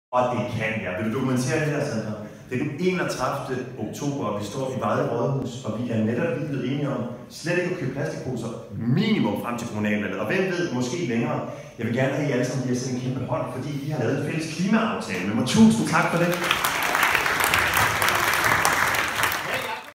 Teater i byrådssalen
Den fine gamle byrådssal i Vejle Rådhus var i går rammen om to teaterforestillinger, da teatergruppen Teatergrad opførte stykket Demokrati, som naturligvis er aktuelt lige nu, hvor kommunalvalget venter lige om hjørnet.